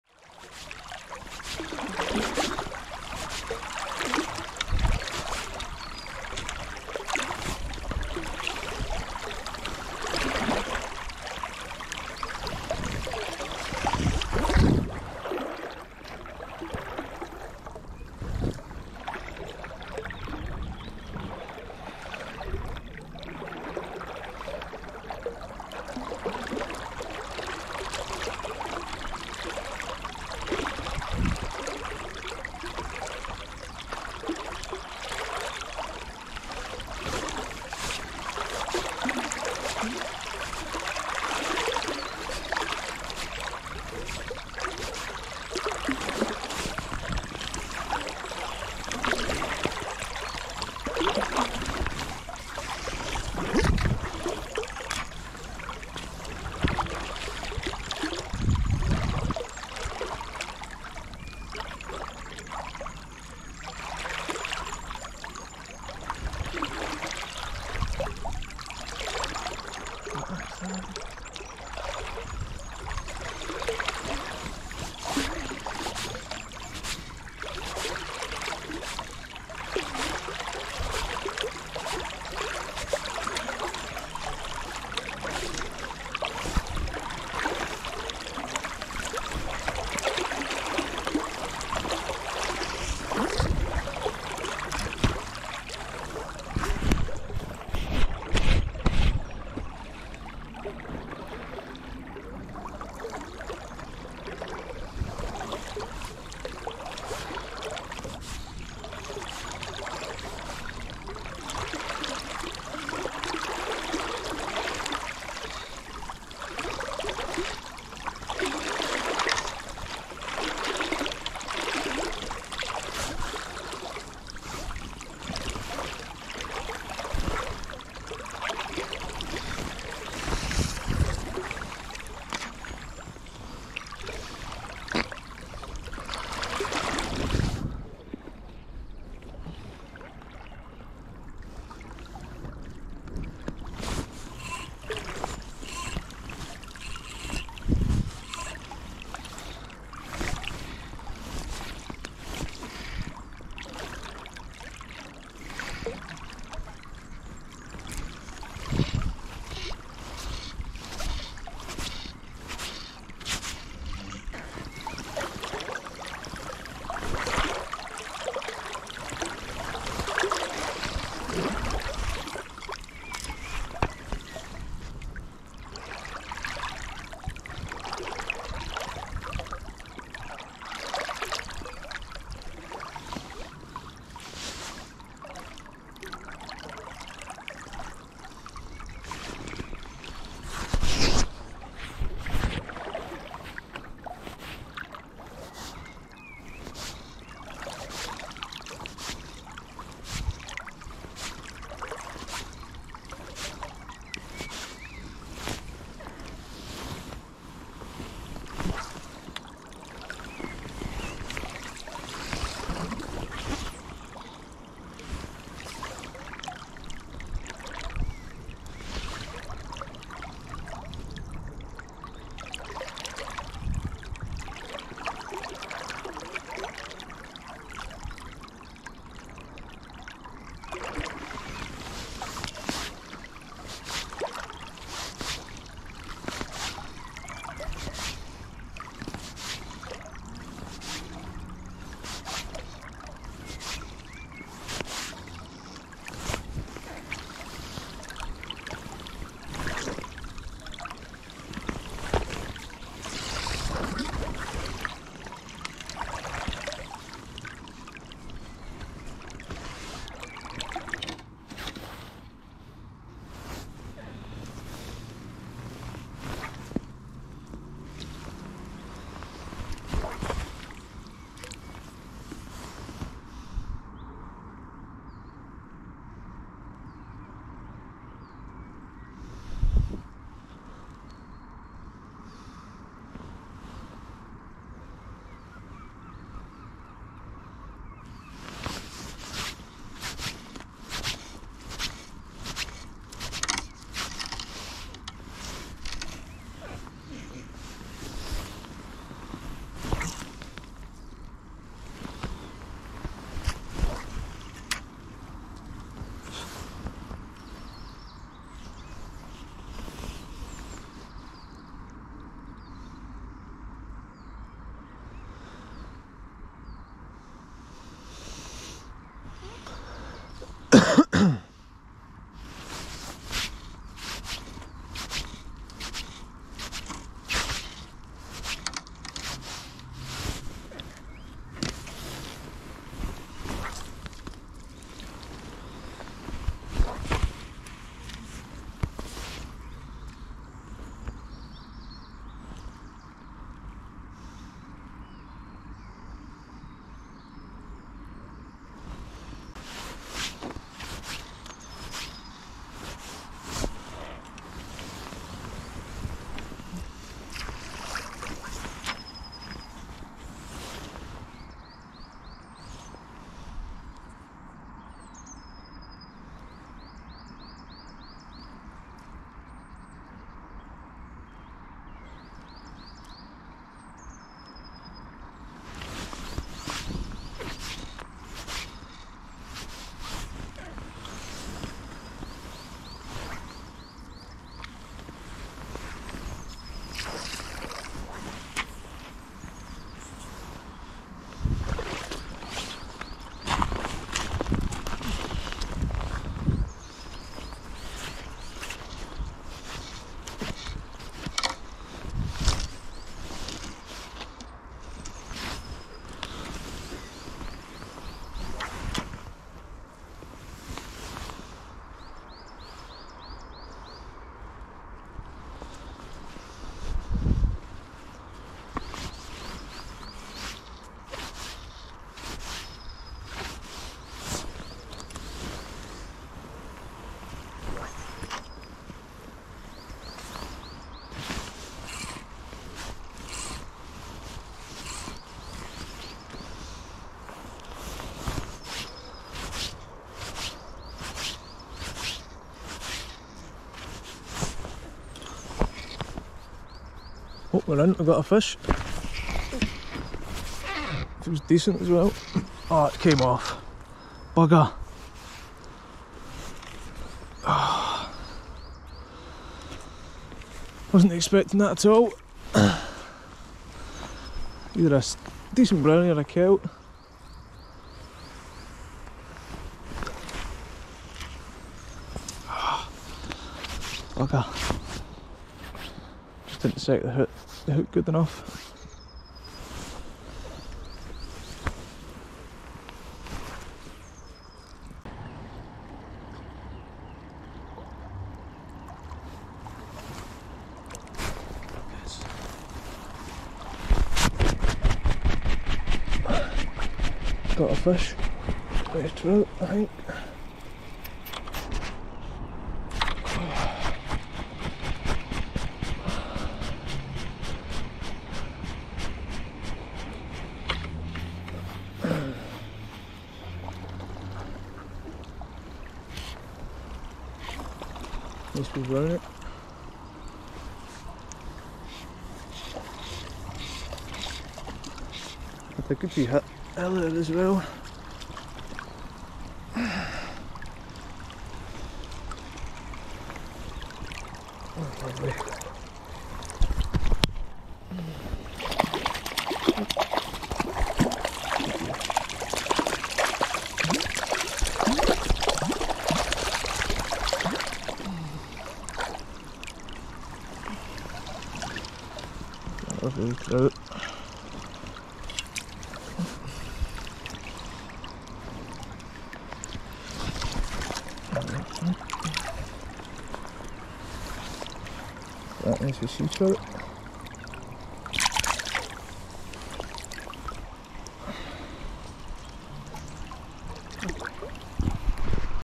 In Search Of Spring Silver
Down for a few casts on the local river hoping to strike lucky and come across a rare spring salmon. It's still considered early for this particular river but you never know there is always a chance.
Hooked a decent kelt and landed a nice little sea trout so was a great day.
Not much talking in this video, hopefully the nice sound of the water is enough to enjoy :) Tight Lines!